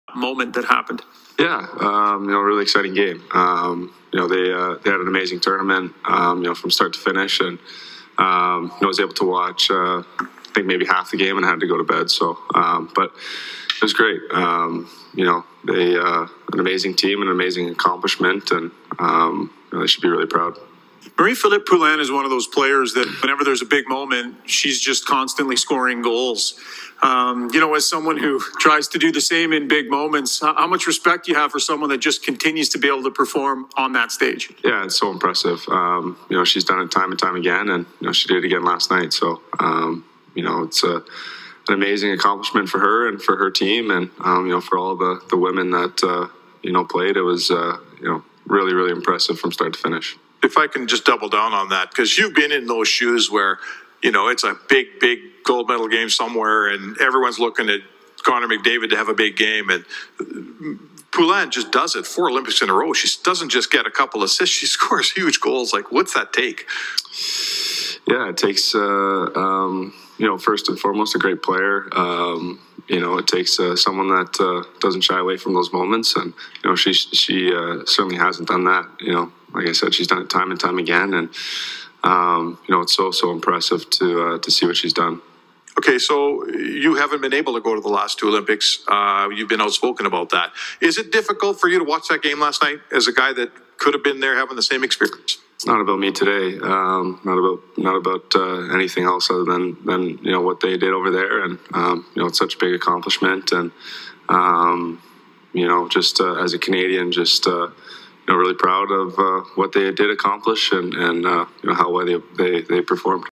Connor McDavid on Marie-Philip Poluin heroics and his thoughts on Women's hockey gold medal !
Connor McDavid talks about team Canada women's hockey gold medal performance